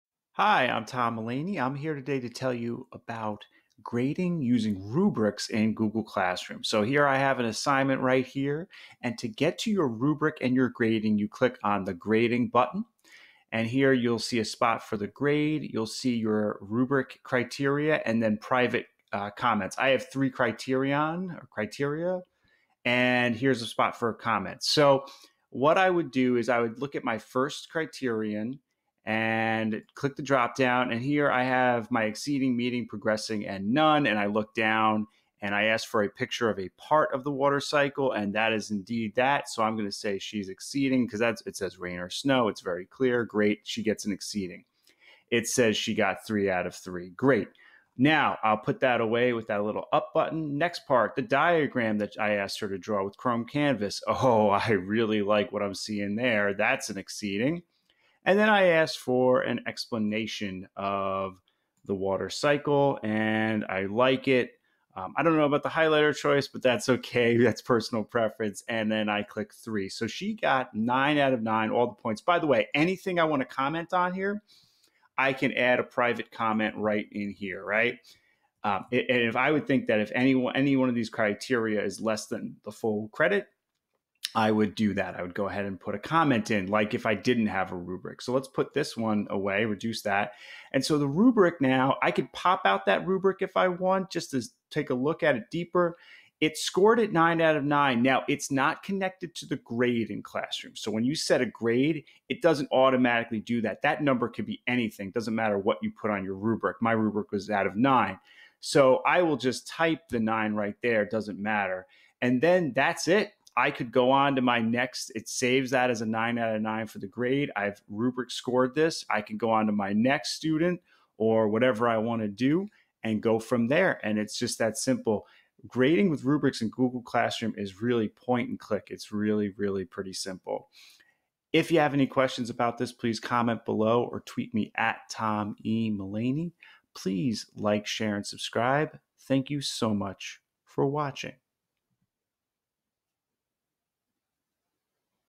Google Classroom - Grading with Rubrics
Grading with rubrics in Google Classroom is as easy as pointing and clicking.